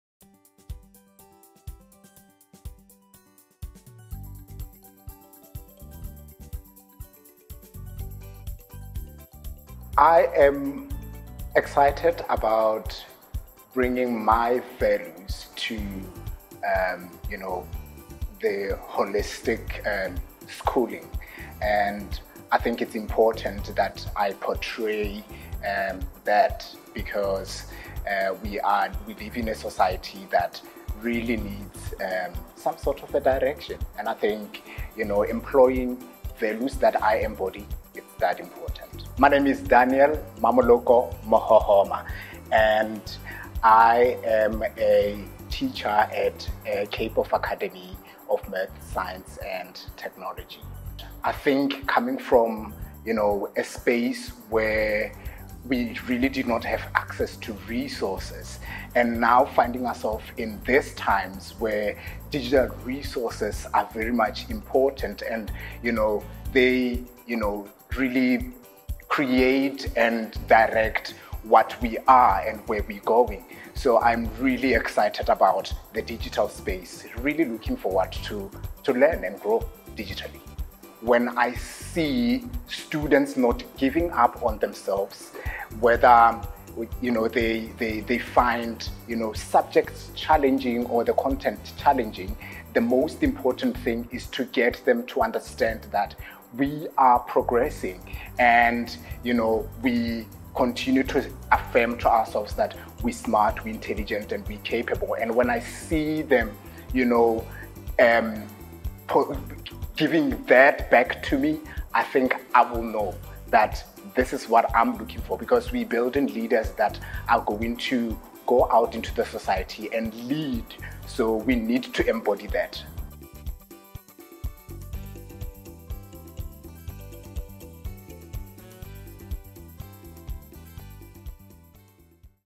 Journey Into The Classroom: Daniel Mohohoma
Daniel Mohohoma is one of our 2022 Jakes Gerwel Candidates. He shares his Journey nto the Classroom with us.